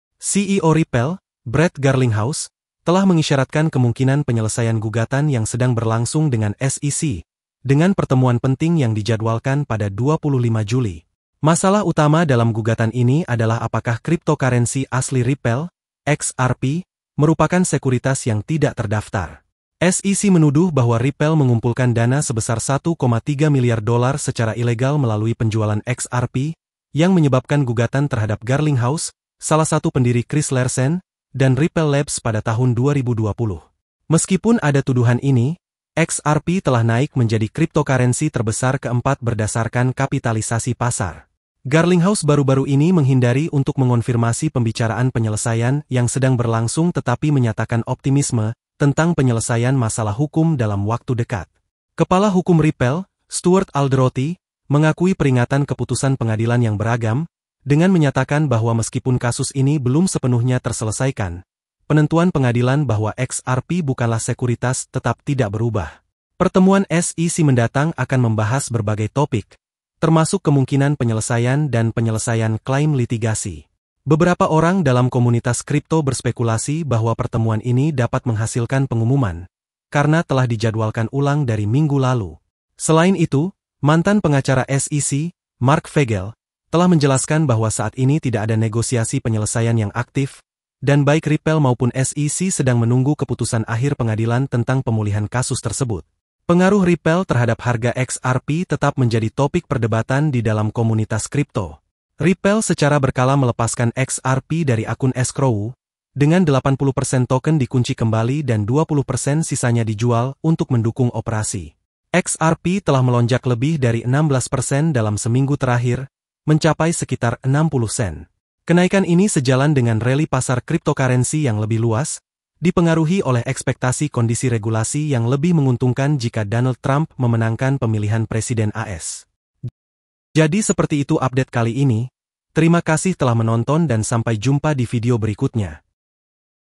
Gugatan Ripple dan SEC Mungkin Akan Diselesaikan pada Pertemuan Tertutup 25 Juli!
CEO Ripple, Brad Garlinghouse, telah mengisyaratkan kemungkinan penyelesaian gugatan yang sedang berlangsung dengan SEC, dengan pertemuan penting yang dijadwalkan pada 25 Juli. Masalah utama dalam gugatan ini adalah apakah cryptocurrency asli Ripple, XRP, merupakan sekuritas yang tidak terdaftar. SEC menuduh bahwa Ripple mengumpulkan dana sebesar $1,3 miliar secara ilegal melalui penjualan XRP, yang menyebabkan gugatan terhadap Garlinghouse, salah satu pendiri Chris Larsen, dan Ripple Labs pada tahun 2020. Meskipun ada tuduhan ini, XRP telah naik menjadi cryptocurrency terbesar keempat berdasarkan kapitalisasi pasar.

Garlinghouse baru-baru ini menghindari untuk mengonfirmasi pembicaraan penyelesaian yang sedang berlangsung tetapi menyatakan optimisme tentang penyelesaian masalah hukum dalam waktu dekat. Kepala Hukum Ripple, Stuart Alderoty, mengakui peringatan keputusan pengadilan yang beragam, dengan menyatakan bahwa meskipun kasus ini belum sepenuhnya terselesaikan, penentuan pengadilan bahwa XRP bukanlah sekuritas tetap tidak berubah.

Pertemuan SEC mendatang akan membahas berbagai topik, termasuk kemungkinan penyelesaian dan penyelesaian klaim litigasi. Beberapa orang dalam komunitas kripto berspekulasi bahwa pertemuan ini dapat menghasilkan pengumuman, karena telah dijadwalkan ulang dari minggu lalu.

Selain itu, mantan pengacara SEC, Marc Fagel, telah menjelaskan bahwa saat ini tidak ada negosiasi penyelesaian yang aktif, dan baik Ripple maupun SEC sedang menunggu keputusan akhir pengadilan tentang pemulihan kasus tersebut.

XRP Melonjak di Tengah Reli Pasar dan Spekulasi Pemilihan

Pengaruh Ripple terhadap harga XRP tetap menjadi topik perdebatan di dalam komunitas kripto. Ripple secara berkala melepaskan XRP dari akun escrow, dengan 80% token dikunci kembali dan 20% sisanya dijual untuk mendukung operasi.

XRP telah melonjak lebih dari 16% dalam seminggu terakhir, mencapai sekitar 60 sen. Kenaikan ini sejalan dengan reli pasar cryptocurrency yang lebih luas, dipengaruhi oleh ekspektasi kondisi regulasi yang lebih menguntungkan jika Donald Trump memenangkan pemilihan presiden AS.